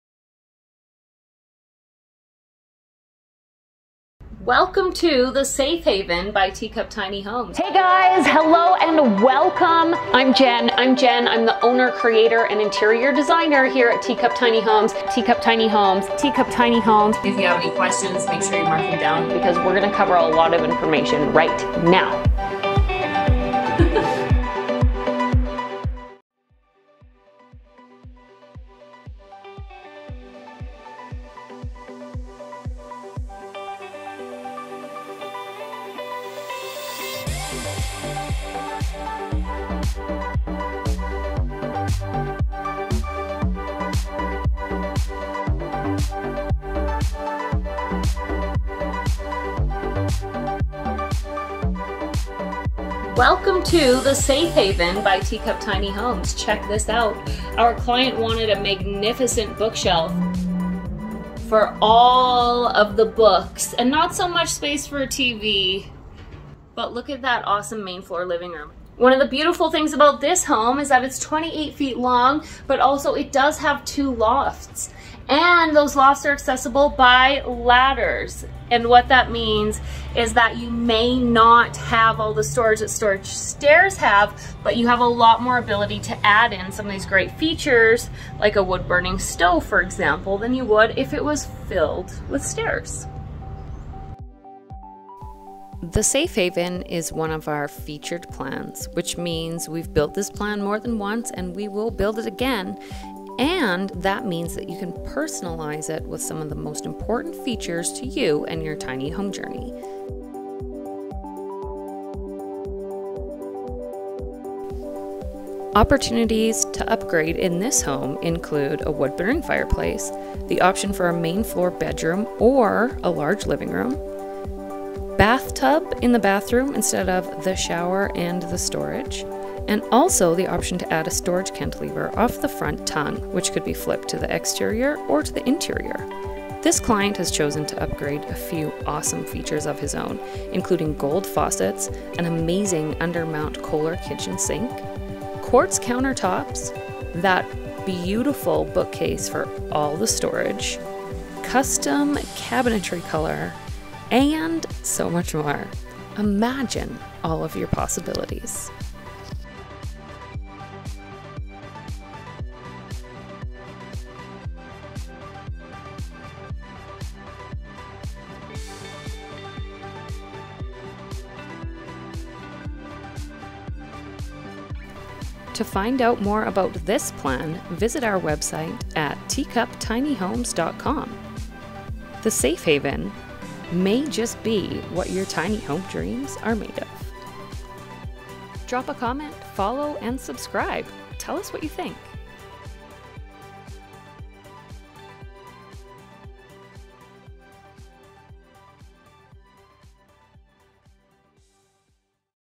STUNNING Double Loft Tiny Home Tour: You don't want to miss this.
The Safe Haven is a featured Teacup plan that can come loaded with options that will make your tiny home dreams come true.  What is to love the most in this tiny?  The wood burning stove, gold faucets, and amazing living room bookshelf to name a few.   What features would you want in your tiny home? Tell us!